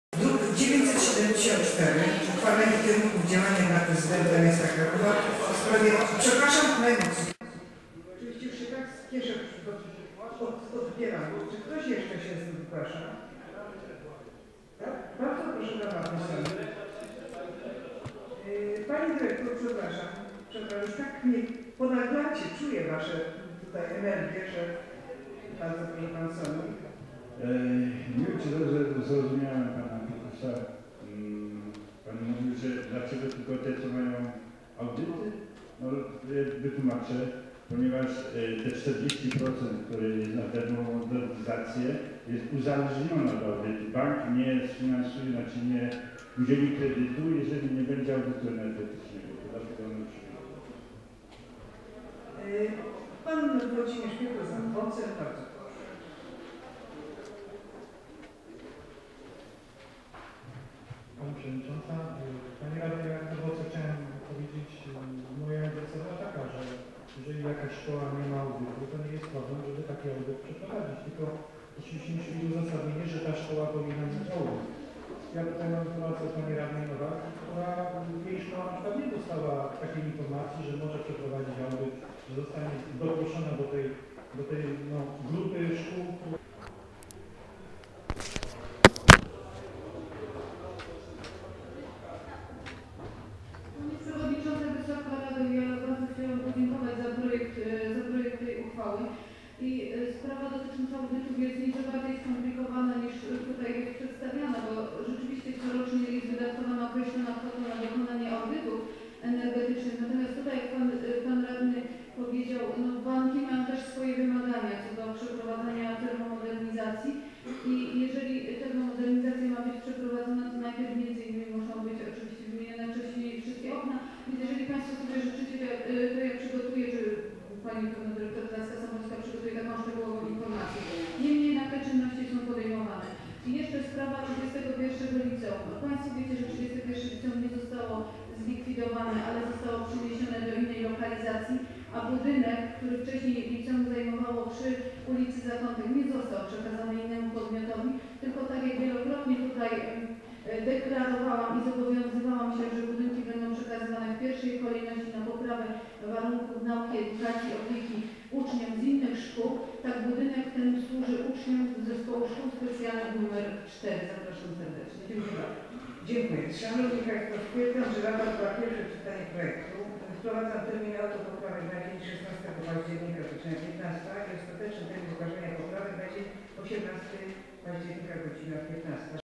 RMK_10-10-2012_52 ws Miejskiego Programu Termomodernizacji Budynków Szkolnych cd
Sesja LVIII Rady Miasta Krakowa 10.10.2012

Ustalenie kierunków działania dla Prezydenta Miasta Krakowa dotyczących opracowania i wdrożenia Miejskiego Programu Termomodernizacji Budynków Szkolnych
Projekt Grupy Radnych - druk nr 973.

Brak video spowodowany brakiem miejsca na karcie pamięci kamery.